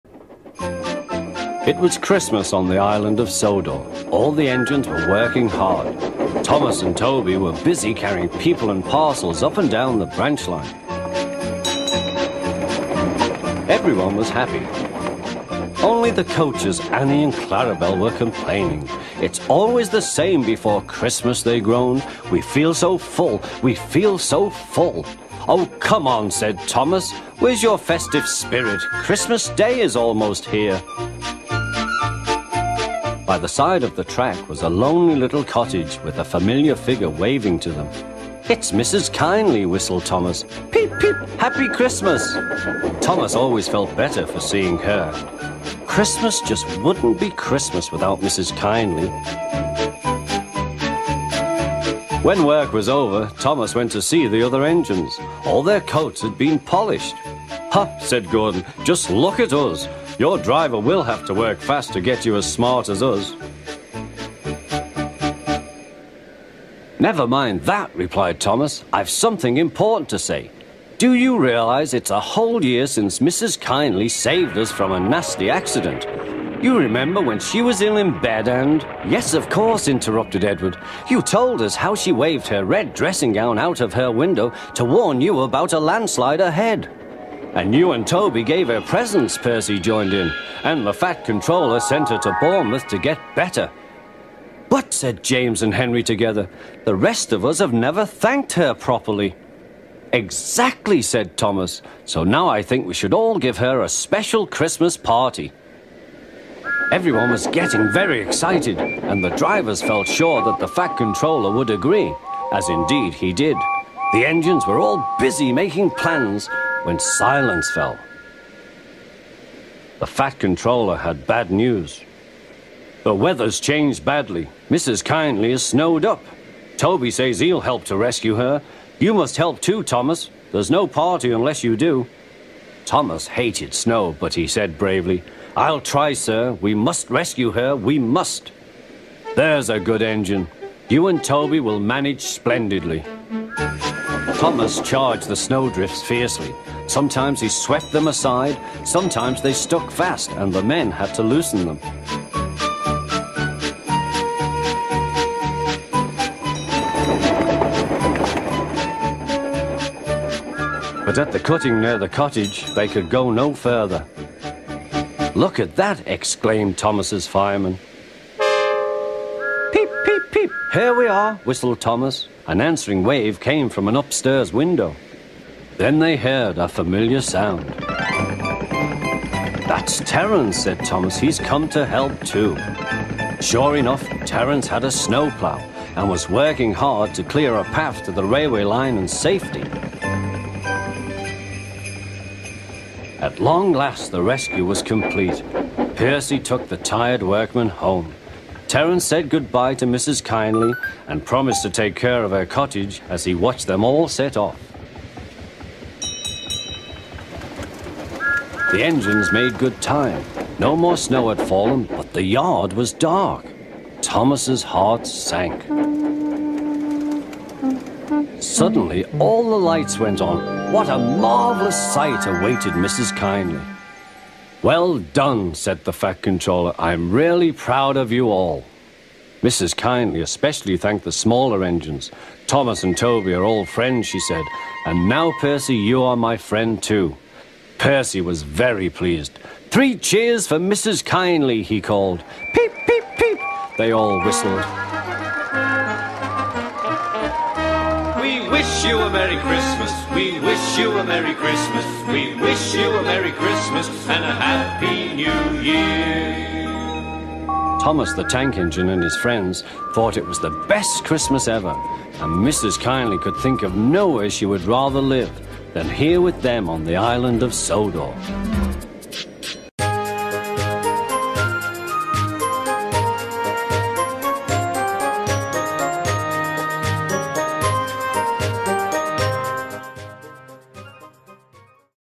Tomy Thomas' Christmas Party (Christmas 2019)
Merry (Late) Christmas everyone! This is the first remake I've created from start to finish in under 24 hours! Hope you all enjoy, and have a Happy New Year!